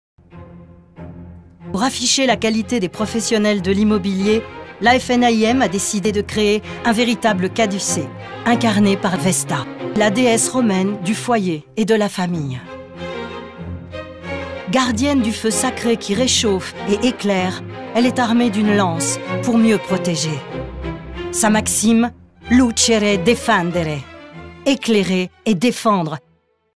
Caen et mer Immo - Les professions immobilières règlementées ont un nouveau symbole, la déesse VESTA
Désormais, reconnaître un professionnel de l'immobilier est chose faisable. Au même titre qu'un notaire, les agents immobilier ont ce caducée.